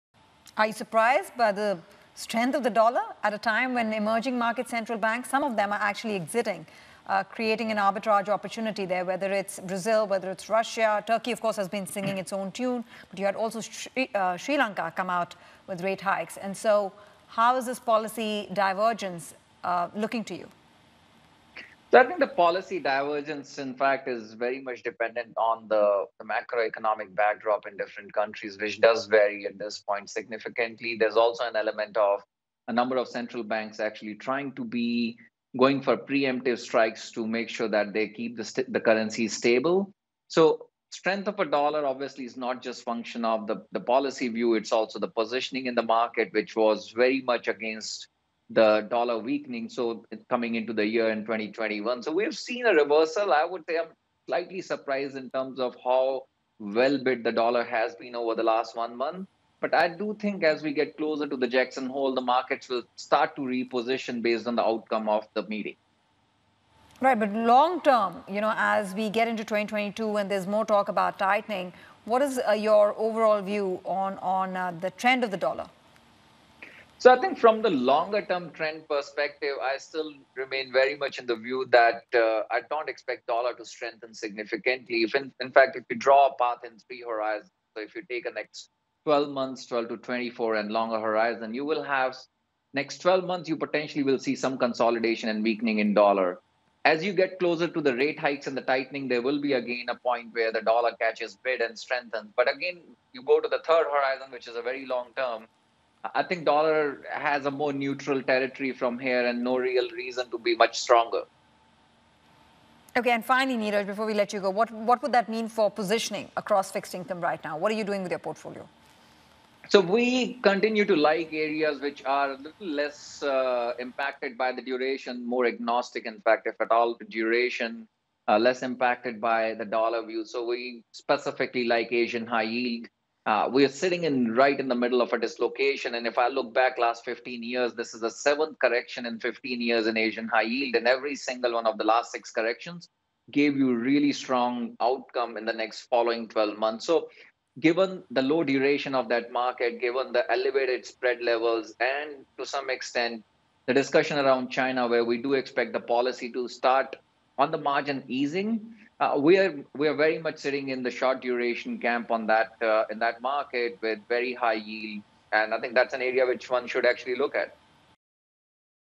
Why BlackRock specifically likes Asian high yield now
Neeraj Seth of BlackRock discusses how he is positioning his fixed income portfolio at the moment.